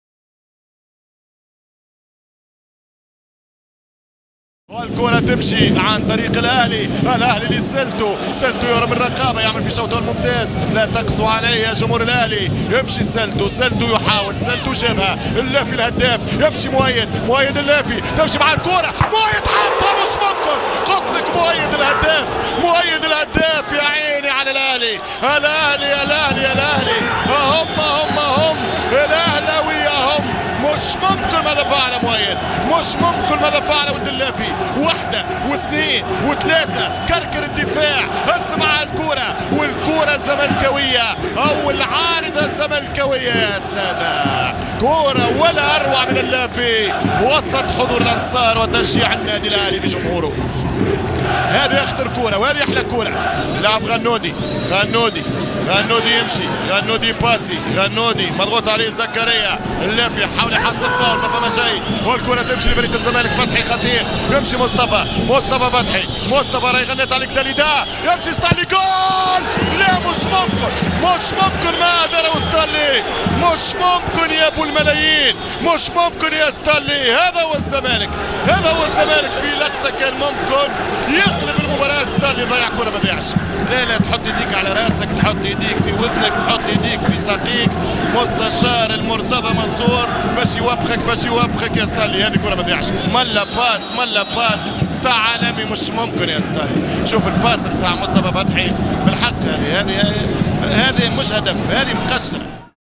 أخطر فرصتين في مباراة الأهلي طرابلس 0-0 الزمالك | تعليق عصام الشوالي | دوري أبطال أفريقيا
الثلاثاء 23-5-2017
CAF Champions League 2017
Al Ahli Tripoli 0-0 Zamalek SC

حقق كل من الاهلي المصري والنجم الساحلي الفوز الثلاثاء في الجولة الثانية من ثمن نهائي دوري ابطال افريقيا لكرة القدم.

في المجموعة الاولى، عاد النجم الساحلي من ام درمان بفوز ثمين على مضيفه المريخ السوداني 2-1.

سجل للفائز ديوغو اكوستا (34) و عامر بوعزة (90+1)، وللخاسر بكري المدينة (52).

كما تعادل فيروفياريو الموزمبيقي مع ضيفه الهلال السوداني في مابوتو سلبا.

ورفع النجم المتصدر رصيده الى 6 نقاط بعد ان تغلب على فيروفياريو 5-صفر في الجولة الاولى، مقابل نقطتين للهلال، ونقطة لك من المريخ وفيروفيارو.

وفي المجموعة الرابعة، حقق الاهلي فوزه الاول وكان على حساب مضيفه كوتون سبور الكاميروني 2-صفر.

وسجل الهدفين العاجي سليمان كوليبالي (12) و مؤمن زكريا (14).

ويلعب الاربعاء زاناكو الزامبي مع الوداد البيضاوي المغربي في لوساكا ضمن المجموعة ذاتها.

وكان الاهلي، صاحب الرقم القياسي في البطولة (7 القاب) وقاريا ايضا (18 لقبا)، تعادل مع زاناكو سلبا في الجولة الاولى التي شهدت خسارة كوتون سبور امام الوداد.

وتصدر الاهلي ترتيب المجموعة برصيد 4 نقاط، مقابل 3 للوداد، ونقطة لزناكو، في حين لقي كوتون سبور خسارته الثانية.

وفي المجموعة الثانية، تعادل الاهلي طرابلس الليبي مع ضيفه الزمالك المصري في تونس سلبا.

ويلتقي الاربعاء في المجموعة ذاتها كابس يونايتد من زيمبابوي مع اتحاد العاصمة الجزائري في هراري.

وفي المجموعة الثالثة، تعادل الترجي التونسي مع مضيفه سانت جورج الاثيوبي سلبا في اديس ابابا فرفع رصيده الى 4 نقاط بعد فوزه في الجولة الاولى على فيتا كلوب الكونغولي 3-صفر.

ويلتقي فيتا كلوب الاربعاء مع صنداونز الجنوب افريقي في كينشاسا.